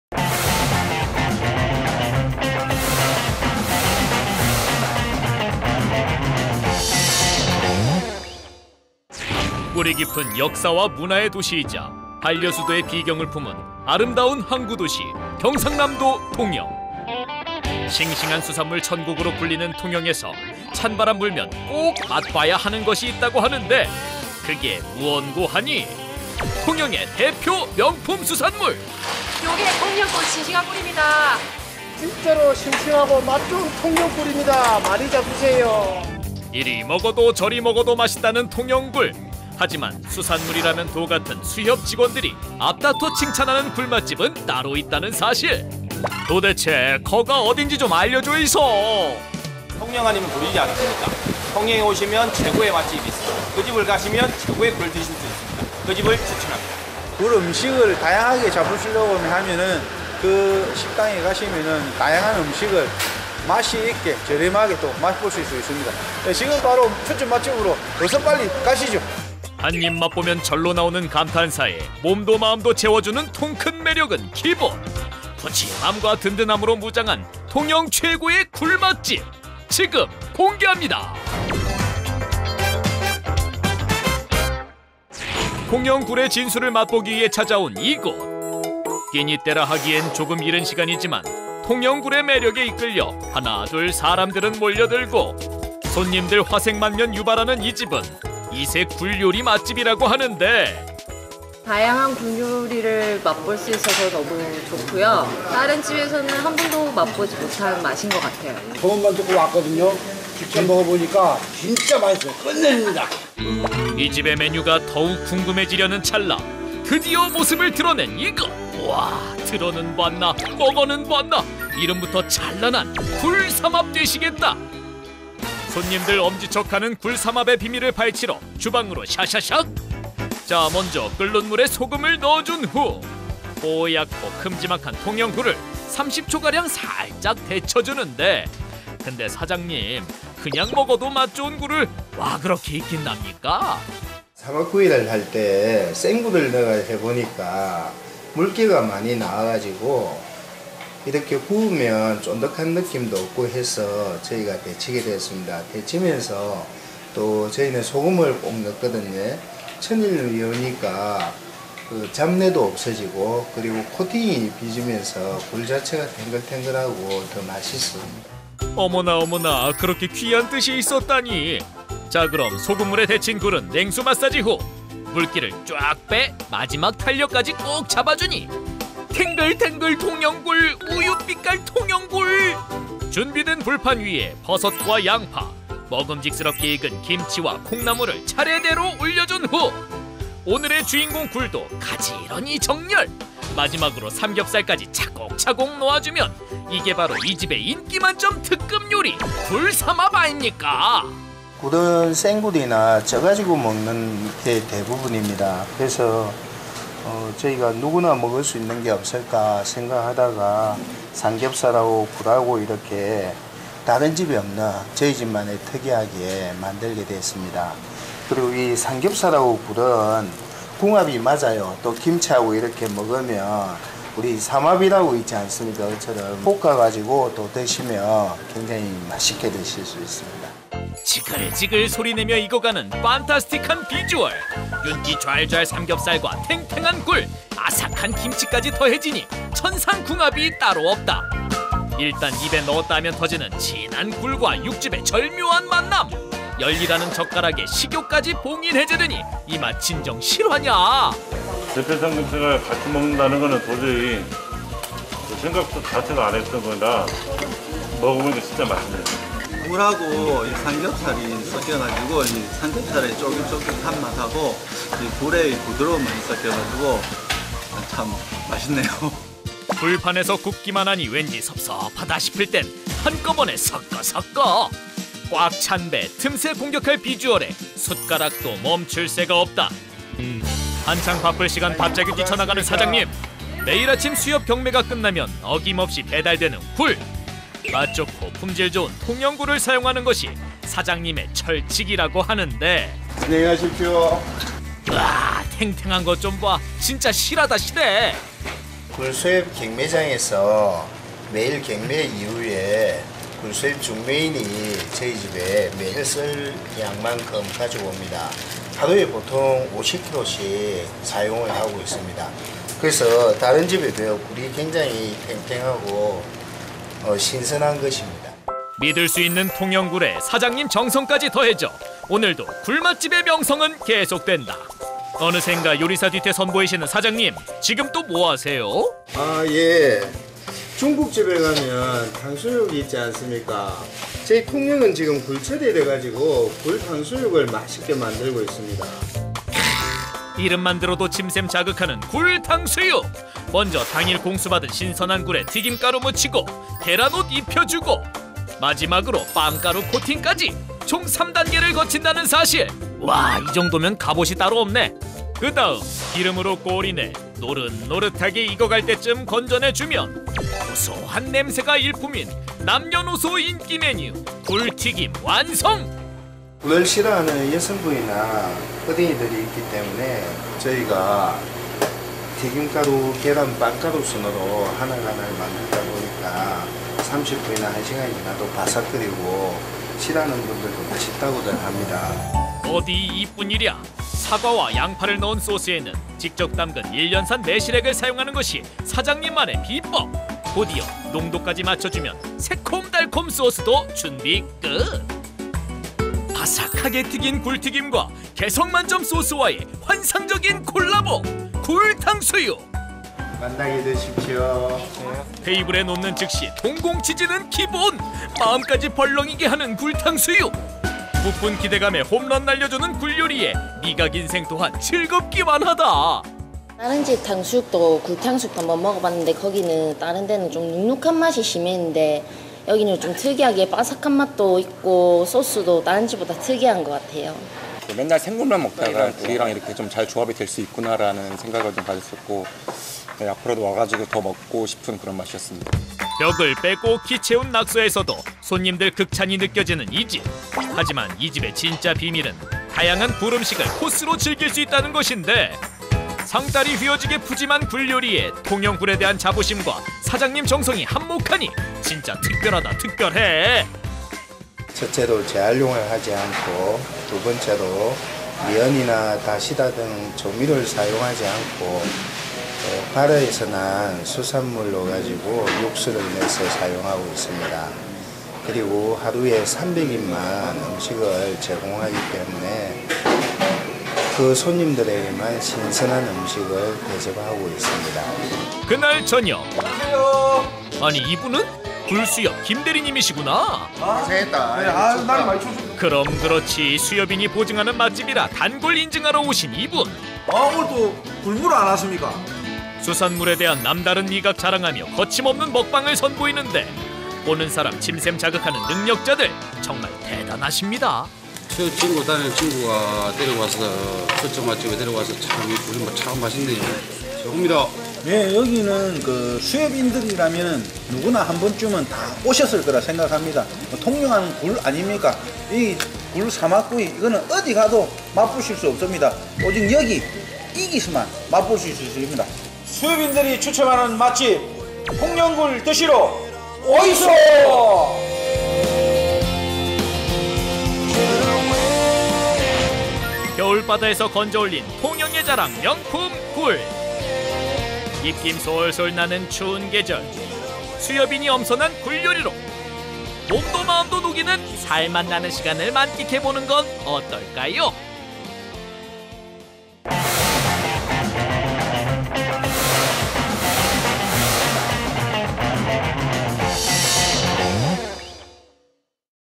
바다옆그집_ 통영굴수협 추천 굴삼합, 굴탕수육 맛집
남해를 대표하는 한려 수도의 비경을 품은 항구도시, 경상남도 통영!
차가운 바람이 불어오는 이때, 살 오르고 맛까지 좋아진다는
통영의 대표 제철수산물은 바로 굴!
뽀얗고 하얀 속살에 큼직한 사이즈는 기본, 입안에서 톡 터지는 담백함과 단맛의 조화가 환상적인 명품 통영굴을 만날 수 있는 맛집이 있다는데...
보는 것만으로도 침샘 자극하는 굴삼합과 바삭하고 고소한 맛이 일품인
굴 탕수육, 게다가 한상 가득 채우는 굴요리의 진수를 맛 볼수 있다는 이집.
수산물에 일가견 있는 수협직원이 보증하는 통영의 진짜 굴 맛집,
바다 옆 그 집에서 그 비밀을 공개한다!